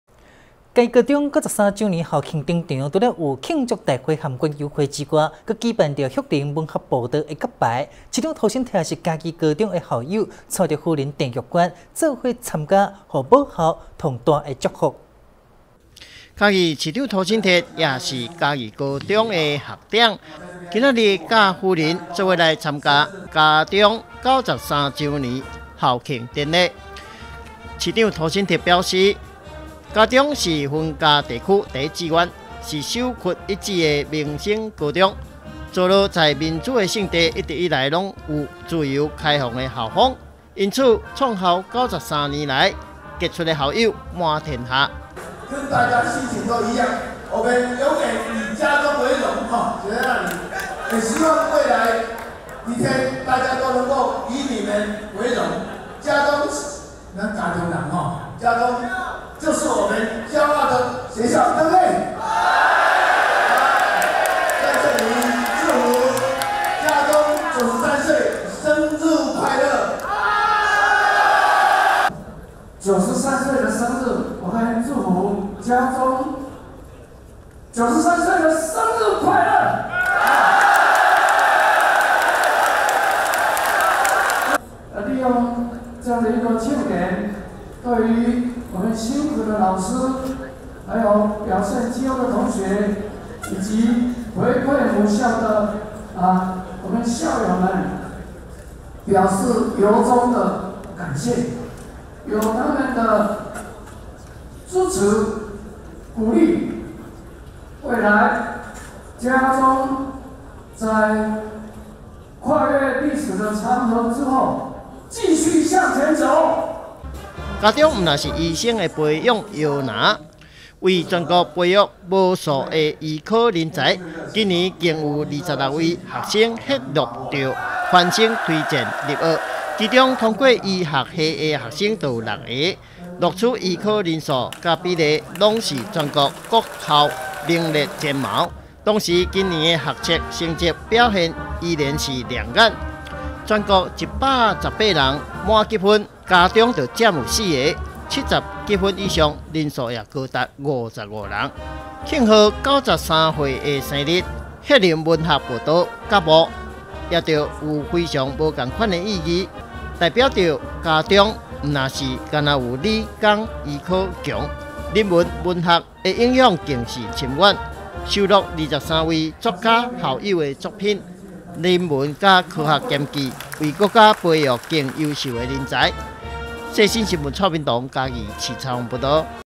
世新新聞 嘉義高中93週年校慶 旭陵文學步道揭牌
106年4月15日(六)世新新聞

嘉義高中93週年校慶 旭陵文學步道揭牌

今(15)日是嘉義高中93週年校慶，除舉行慶祝大會與園遊會之外，更辦理「旭陵文學步道」揭牌啟用典禮，也是嘉中校友的市長涂醒哲偕同夫人鄭玉娟一同參加給予母校最大祝福。